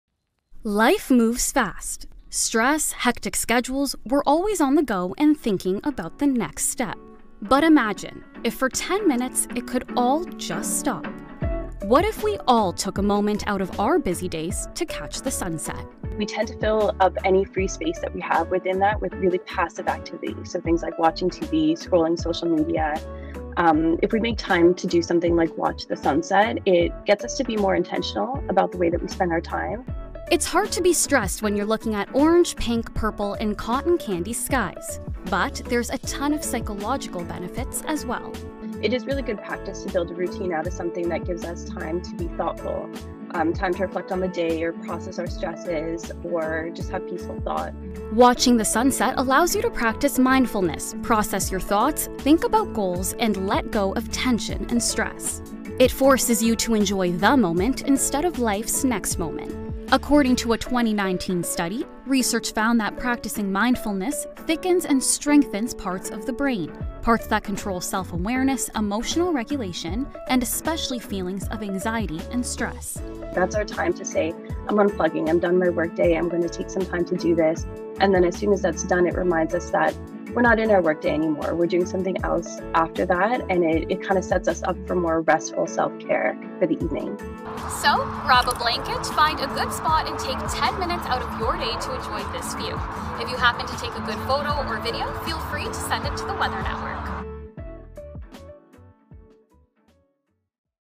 Why this doctor wants you to watch tonight's sunset
Mental health continues to be at the forefront of conversation. According to psychotherapist Zuwaina Murad, watching the sunset for even a short period of time has a number of positive benefits.